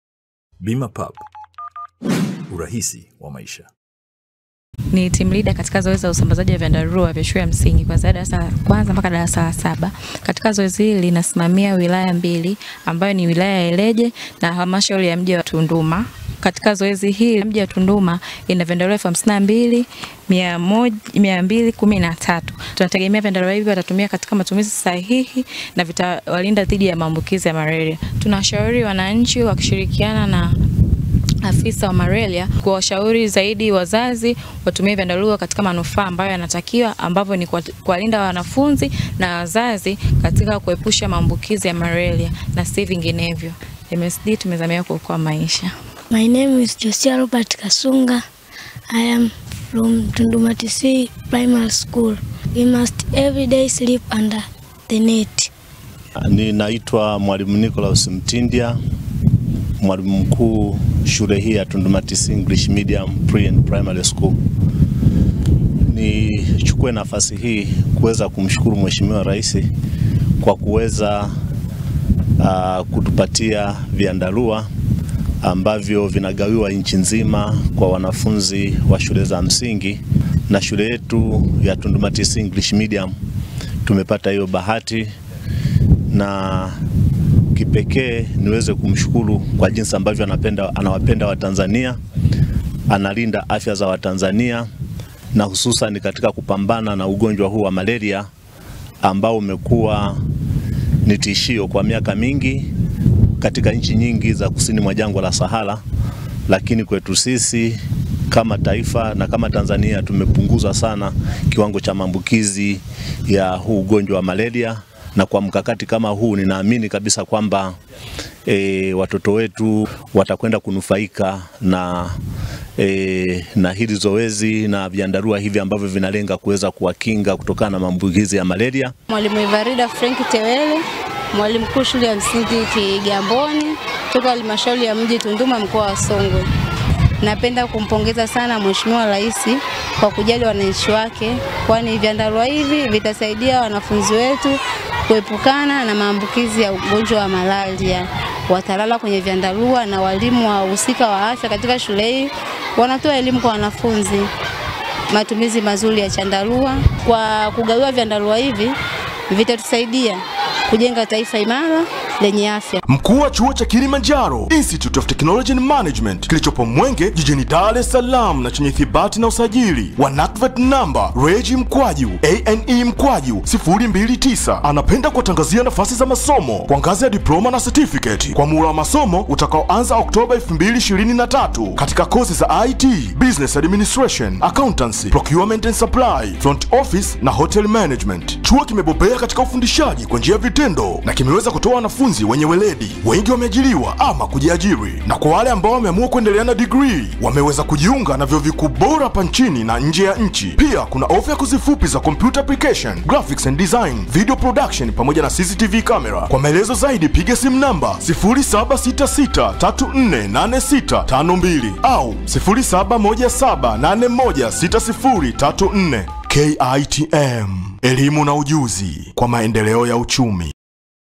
SERIKALI KUPTIA MSD YASAMBAZA VYANDARUA KWA WANAFUNZI, HALMSHAURI YA MJI TUNDUMA...
Leo tarehe 01/11/2023 Bohari ya Dawa(MSD) imegawa  vyandarua 52,000 kwa  wanafunzi wa Darasa la kwanza hadi la Saba  katika shule 66 za Halmshauri ya Mji Tunduma mkoani Songwe kwa lengo la kupambana na  Maambukizi ya Malaria kuelekea kipindi cha Mvua za masika ambacho Mbu huzaliana kwa wingi.

Akiwa katika Halmshauri ya mji Tunduma msimamizi wa zoezi wilaya za Ileje na Tunduma Maryroyd Mike Mdachi amesema kuwa lengo la ugawaji huo ni kutokomeza ugonjwa wa Malaria kwa kuwakinga wanafunzi na Mbu waenezao malaria kwa kuwafunika vyandarua.

Mdachi amewataka wazazi wa wanafunzi kuvitumia vyandarua hivyo kwa lengo lililokusudiwa na kuacha kutumia vyandarua katika bustani za kilimo na matumizi mengine yasiyokusudiwa.

Akipokea vyandarua hivyo kwa niaba ya wanafunzi Mwl Nicolaus Mtindya wa shule ya Msingi Tunduma Tc amesema kuwa kama walimu wanatoa elimu ya Matumizi sahihi ya vyandarua kwa wanafunzi ili wawe  uwezo wa kuvitumia wenyewe pindi wazazi wanaposahau kuwafunika.